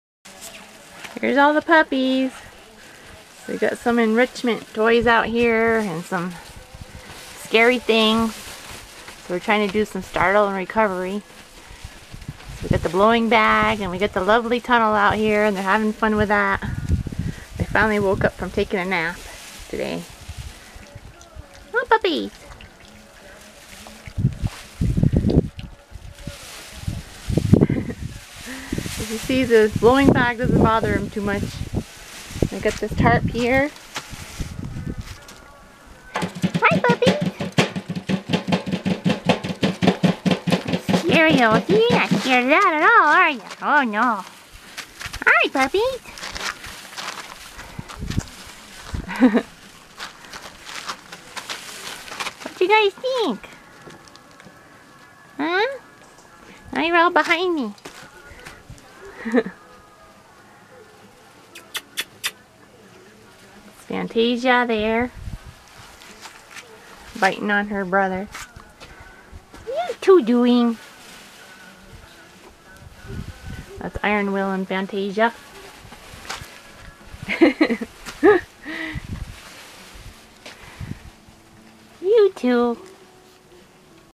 Prospect Pyrenees 4 wks Enrichment Effect
Puppies getting lots of enrichment including down n dirty excercise